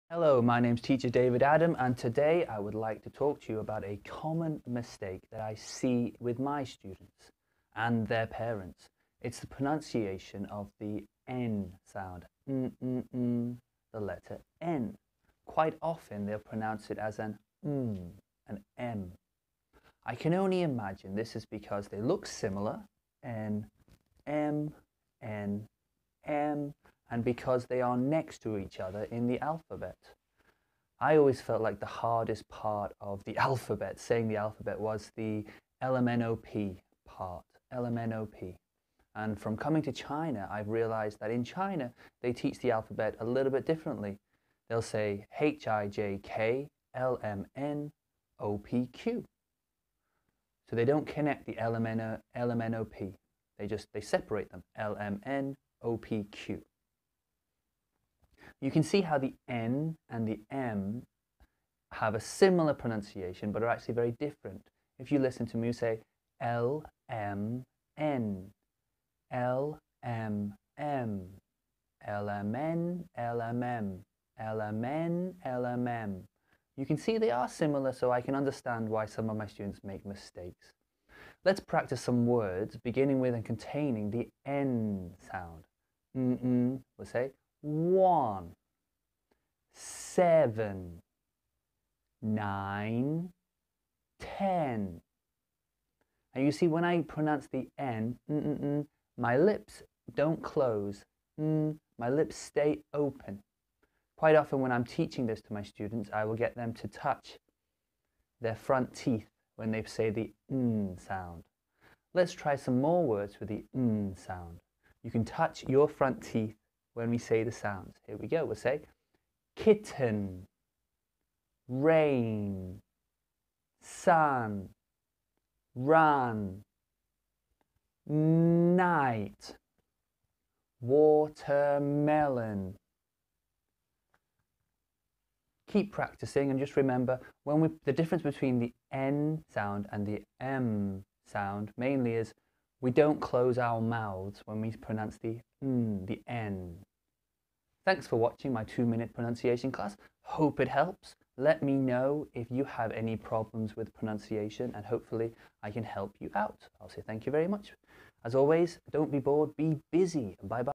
Common Pronunciation problems /m/ and /n/
A common mistake which I see my students as well as their parents make is the correct pronunciation of the /n/ sound. Often they pronounce an /n/ as an /m/. Here I talk about some of the reasons I think this might be the case and what we can do to make sure we pronounce the /n/ sound properly.